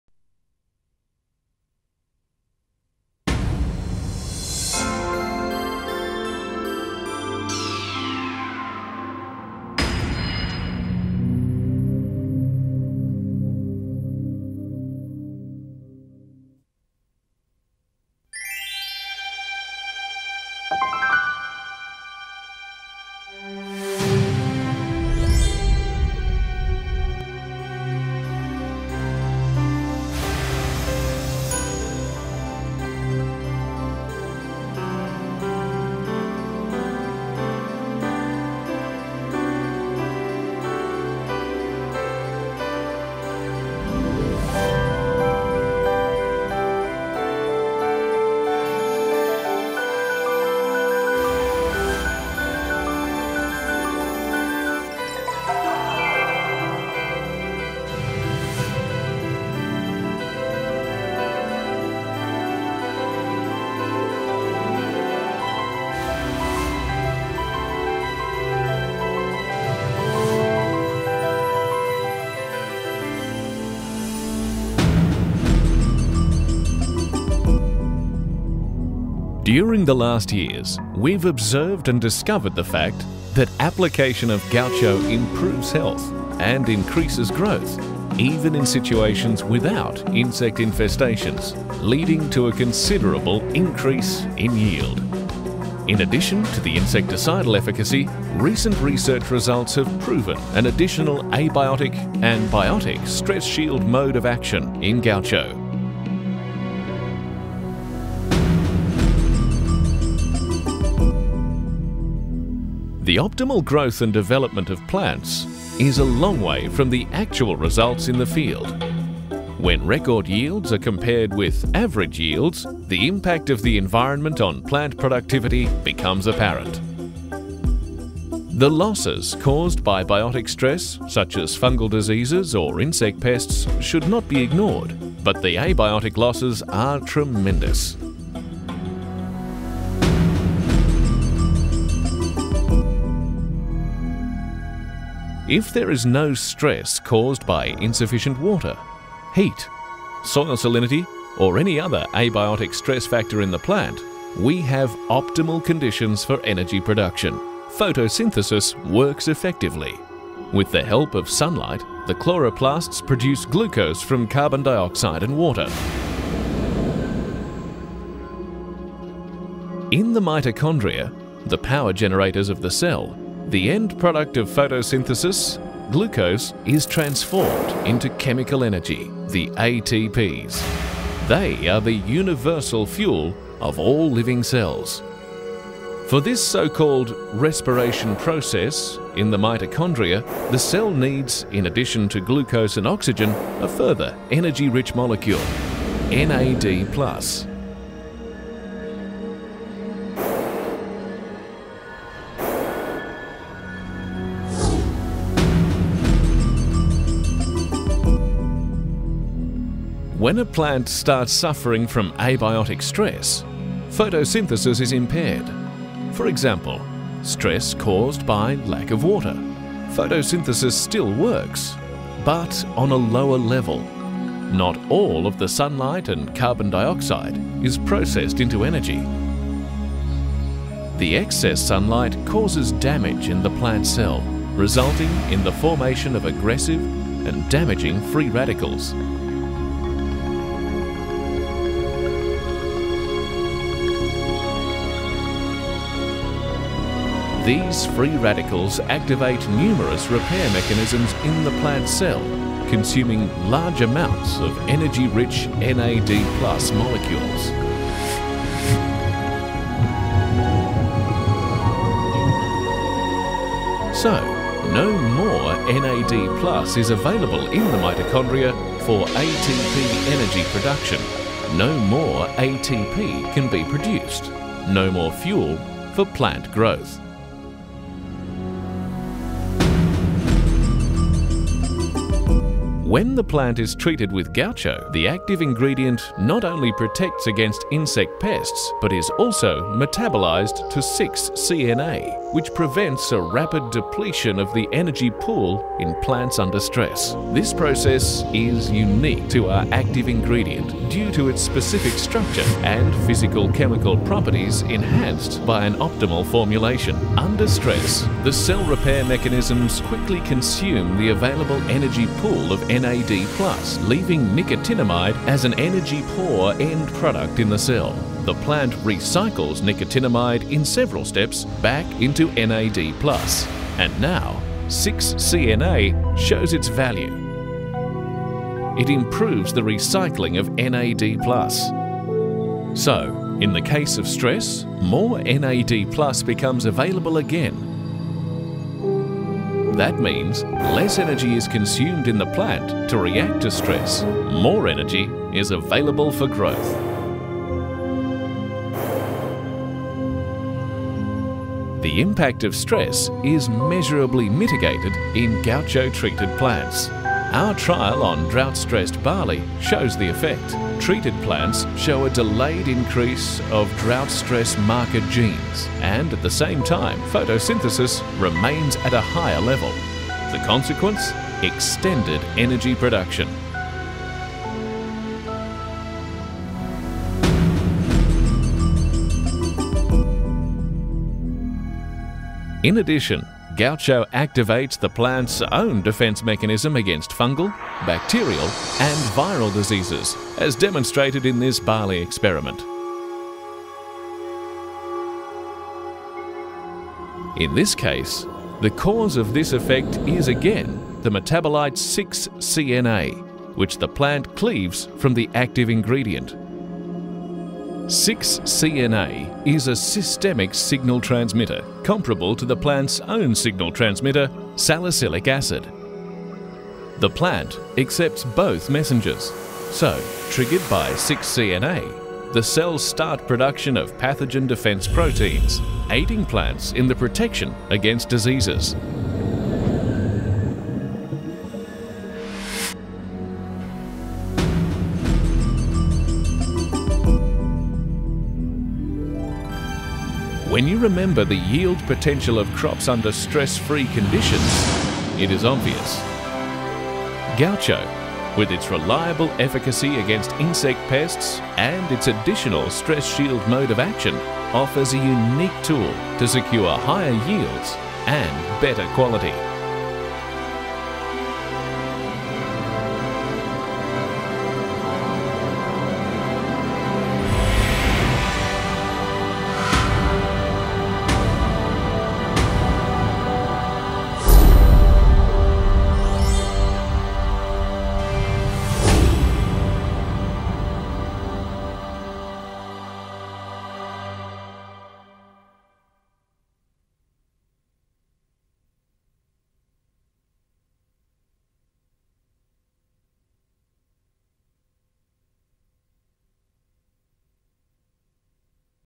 Bayer CropScience - Gaucho + Stress Shield
Learn about Gaucho's second mode of action that has the potential to improve plant health and increase growth, leading to improved yields.